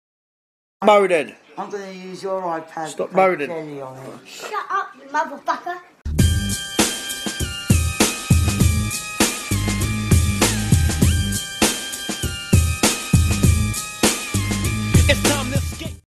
Shut Up You Mother******
He is a bit disrespectful but i did not teach him these words... Okay maybe I did :b